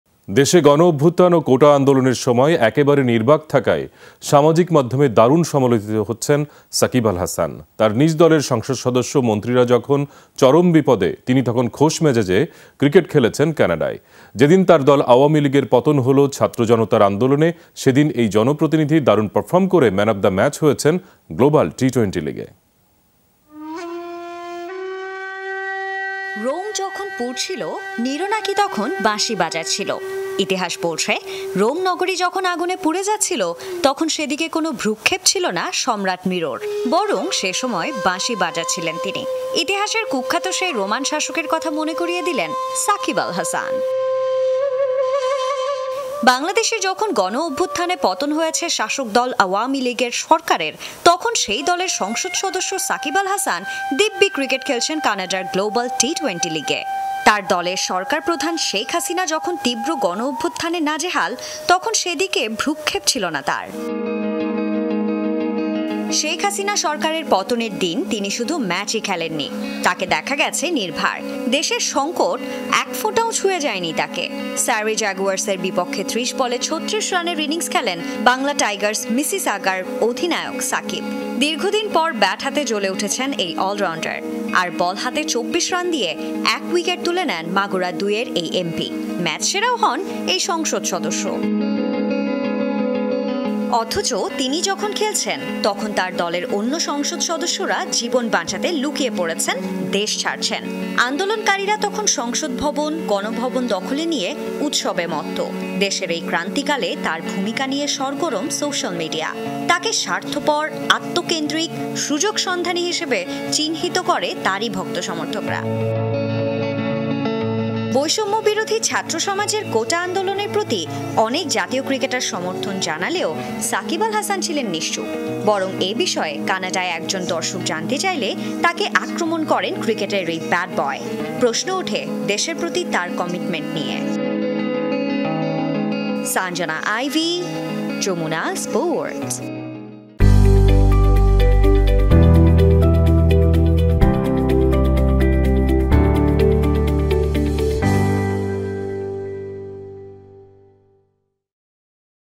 আওয়ামী লীগ এমপিরা যখন দিশেহারা, সাকিব তখন মশগুল ক্রিকেটে! | Shakib Al Hasan | Jamuna TV
আধুনিক যুগের নিরো!

সাকিবের ছবি/নিরোর ছবি

Address:
Bangladesh.